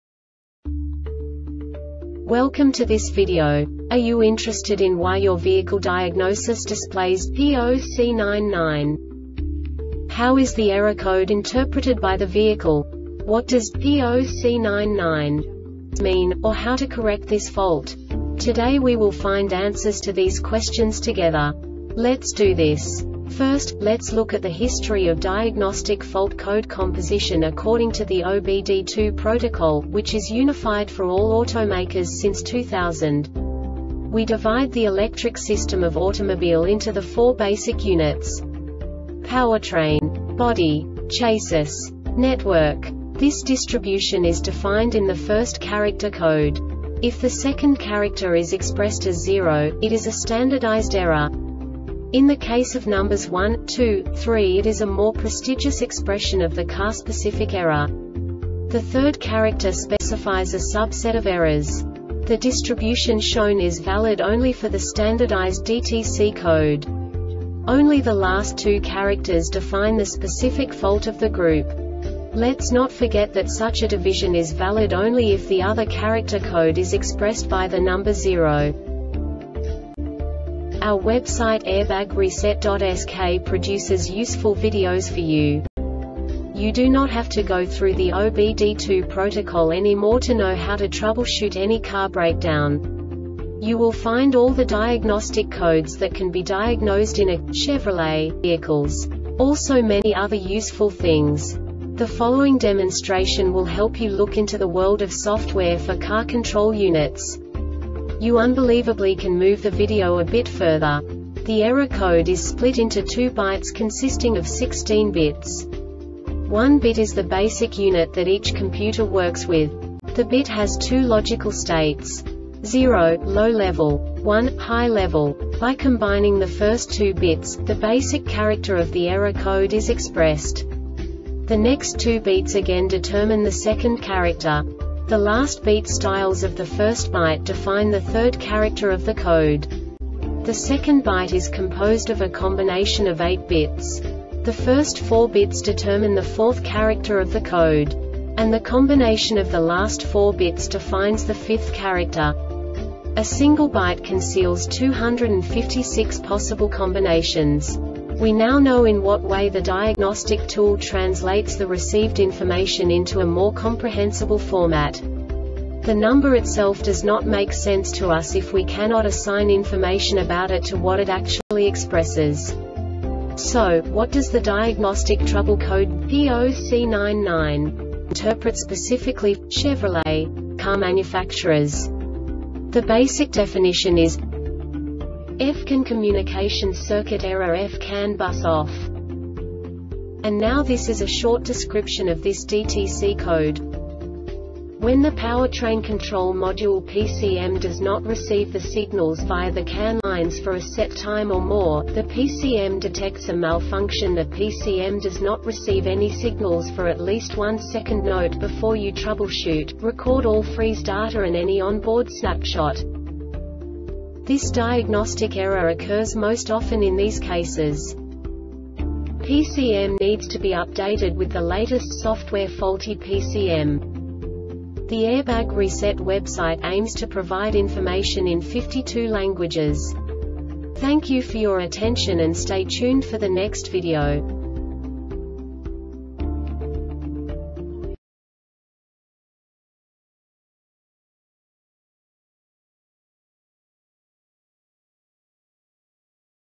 DTC Chevrolet P0C99 Short Explanation
Contents:
0:21 Basic DTC analysis according to OBD2 protocol standard.
1:48 Insight into programming
2:58 A diagnostically understandable description of the Diagnostic Trouble Code
3:05 Basic error definition

Make:
Chevrolet
Code:
P0C99
Definition:
F-CAN Communication Circuit Error (F-CAN Bus OFF)
Description:
When the powertrain control module (PCM) does not receive the signals via the CAN lines for a set time or more,the PCM detects a malfunction.The PCM does not receive any signals for at least 1 second.NOTE: Before you troubleshoot,record all freeze data and any on-board snapshot.
Cause:
 PCM needs to be updated with the latest software Faulty PCM